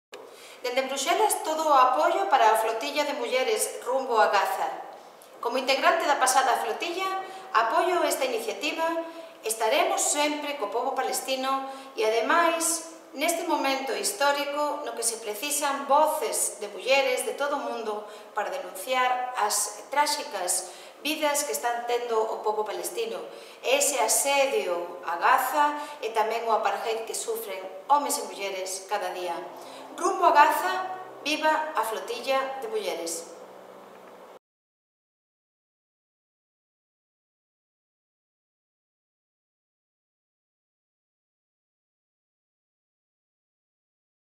Ana Miranda (BNG) apoya Mujeres Rumbo a Gaza
Spanish MEP Ana Miranda endorses Women's Boat to Gaza.
El eurodiputado español Ana Miranda apoya Mujeres Rumbo a Gaza